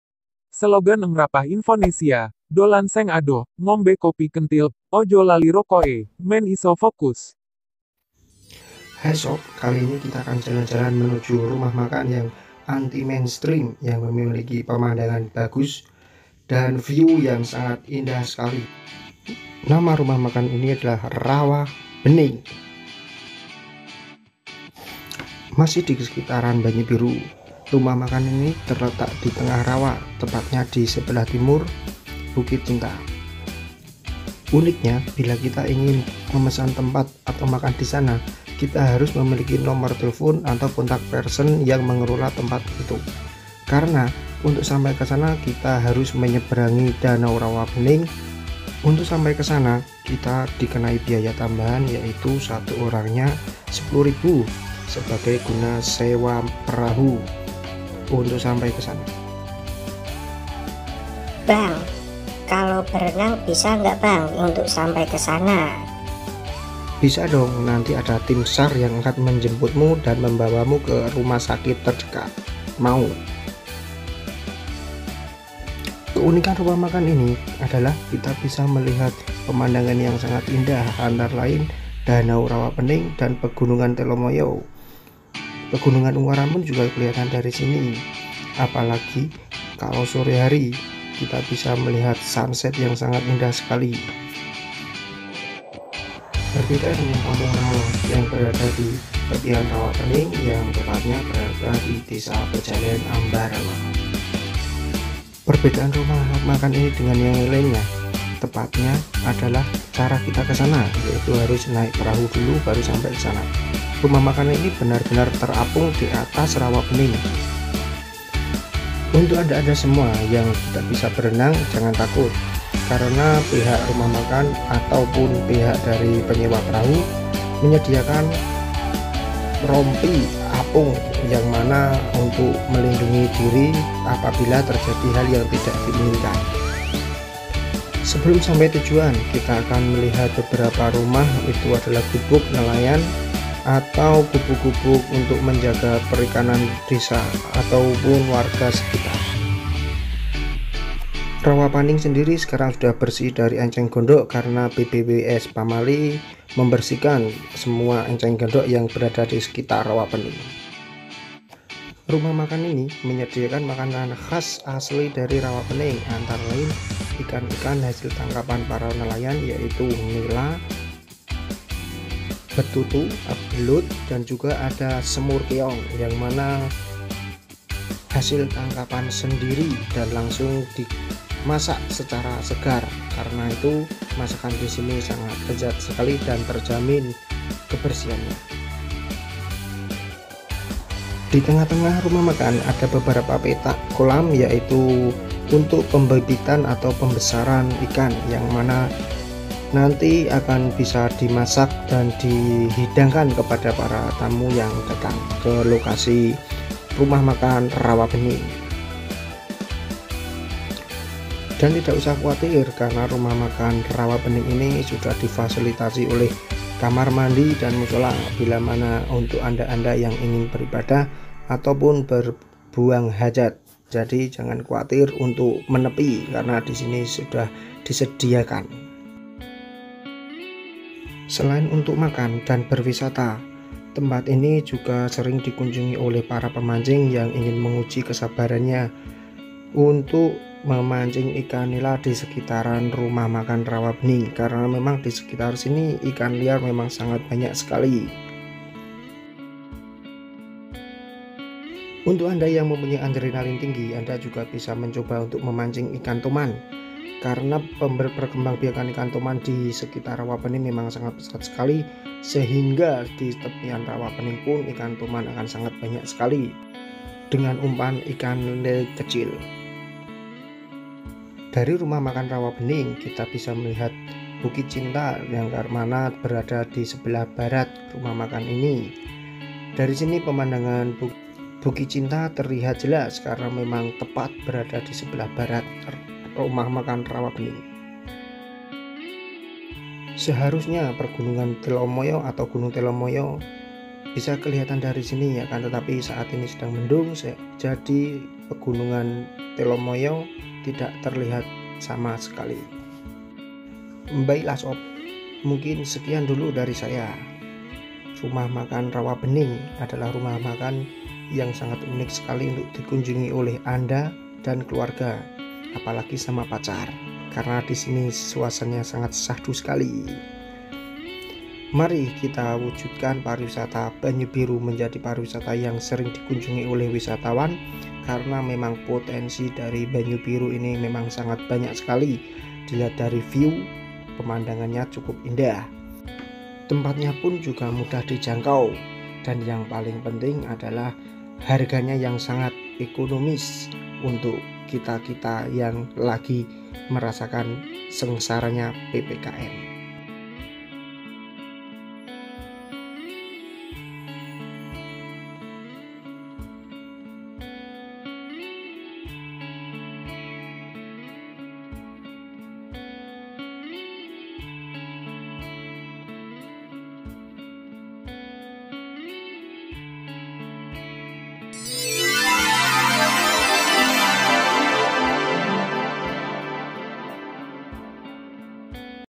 RAWA PENING , RUMAH MAKAN UNIQ " RAWA BENING "
rumah makan rawa bening rumah makan yang berada di banyubiru dan di tengah rawa.